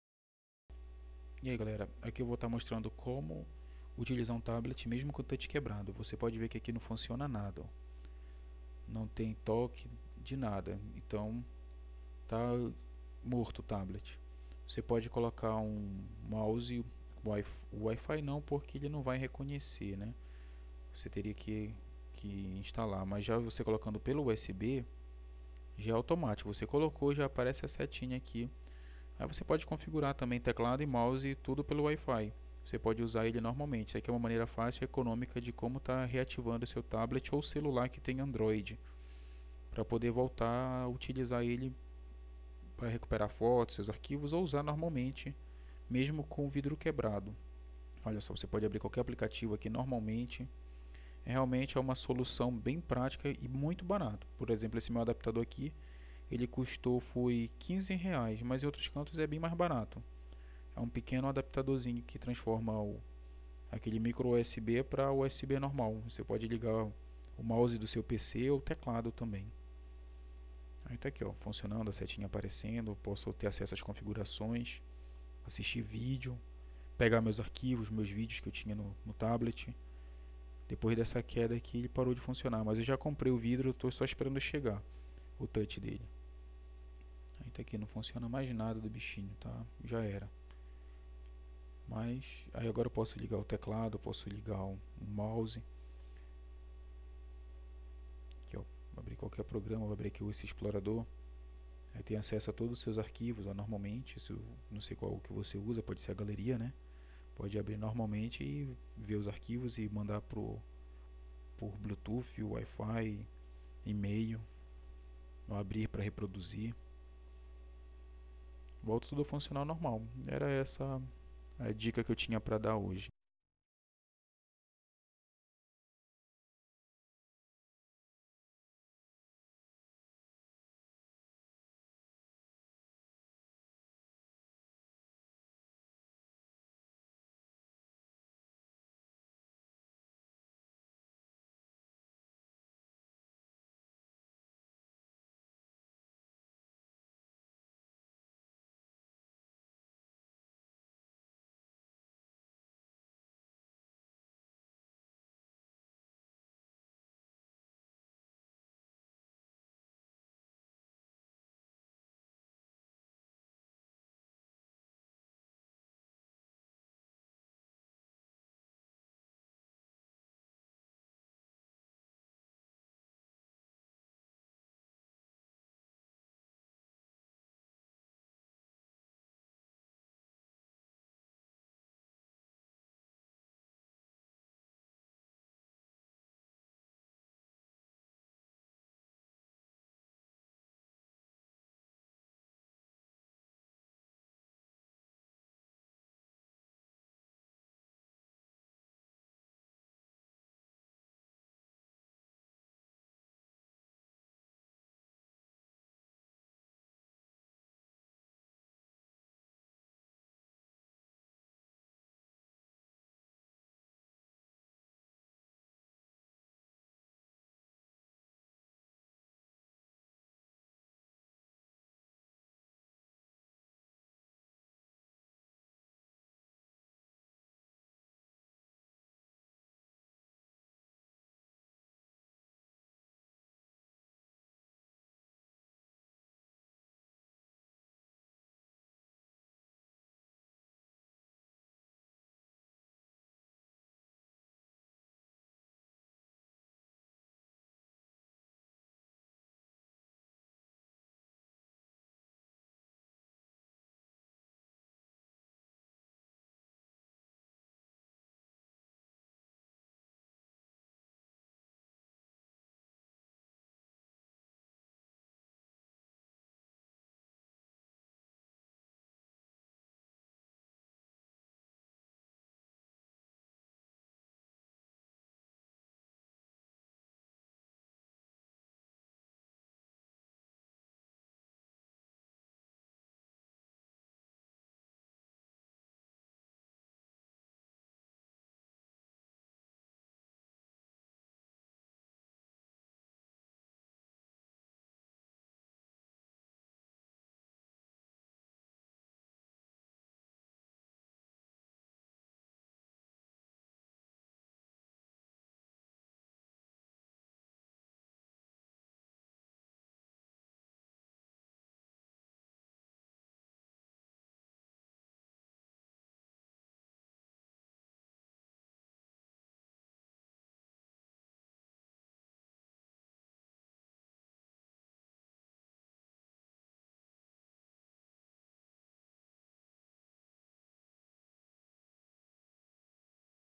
Como usar o tablet com a tela quebrada - Sem trocar o vidro
meu tablet quebrou o touch ai testei com um mouse e pegou
confira no video

nesse video mostro que da p usar seu tablet com o touch quebrado serve para quase todos dispositivos android basta ligar um mouse ou teclado usb e pronto! testei em um tablet samsung   nao pegou, M 7 SDual core, e em um multlaser e um prestigio navcity nt 1711 pegou, esse do video e um tablet ht704 how
se nao pegar no seu avise pra colocar na lista dos que não pega.
avise se não pegar no seu tablet.